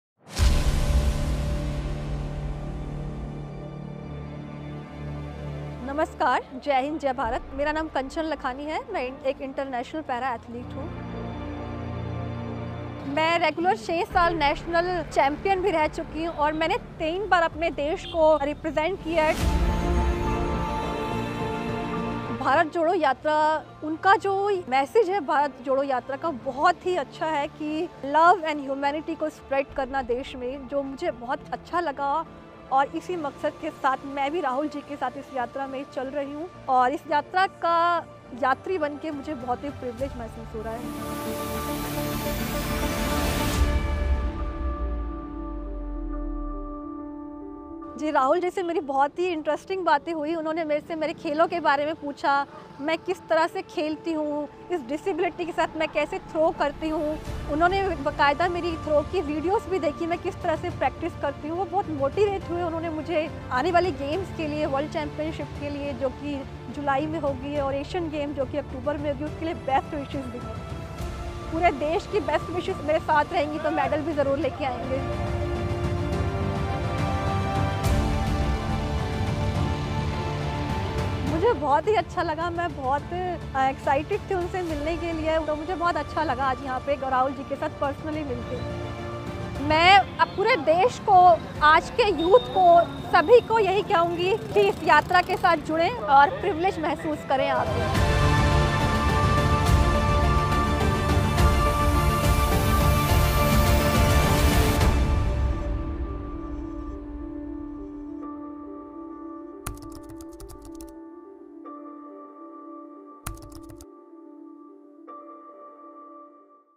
'Bharat Jodo Yatra का संदेश प्रेम, सद्भाव के साथ रहकर समाज को एकजुट रखना है'...
BharatJodoYatra का संदेश प्रेम, सद्भाव के साथ रहकर समाज को एकजुट रखना है, देश को एक रखना है और नफरत को हराना है। इसी संदेश ने अंतर्राष्ट्रीय पैरा एथलीट कंचन लखानी जी को प्रभावित किया और वह यात्रा के साथ जुड़ गई।